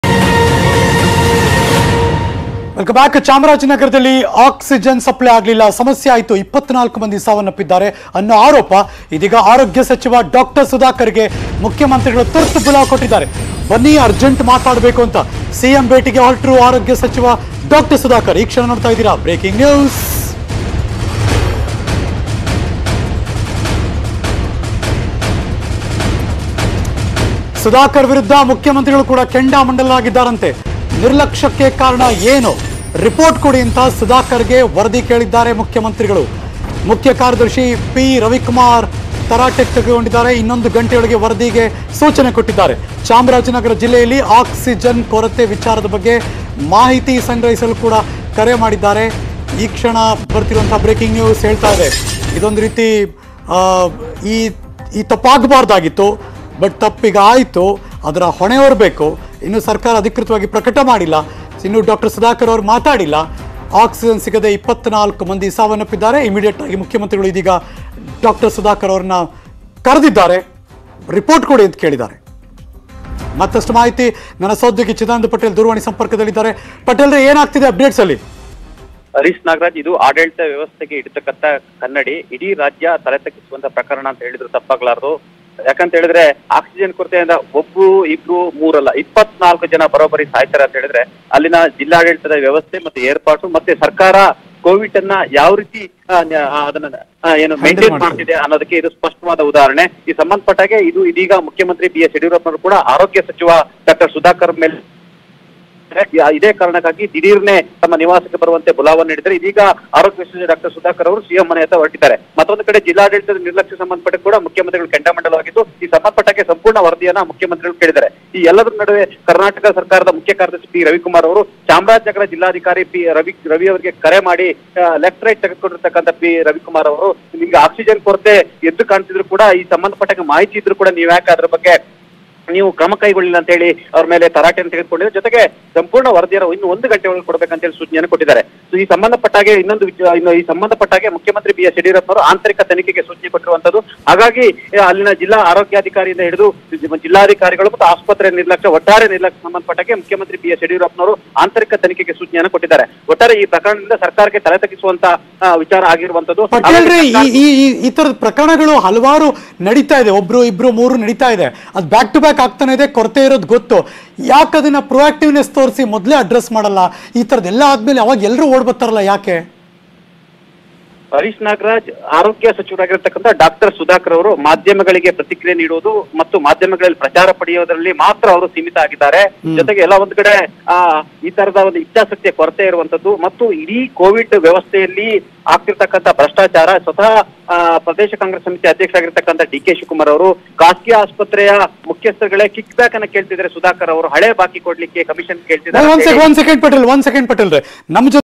Chamarajanagarದಲ್ಲಿ Oxygen ದುರಂತ; ಆರೋಗ್ಯ ಸಚಿವ K Sudhakarಗೆ CM ತುರ್ತು ಬುಲಾವ್
Chamarajanagarದಲ್ಲಿ 24 ಜನ ಸಾವಿನ ಆರೋಪ. K Sudhakarಗೆ CM ತುರ್ತು ಬುಲಾವ್. BS Yediyurappa ಭೇಟಿಗೆ ಹೊರಟ ಆರೋಗ್ಯ ಸಚಿವ.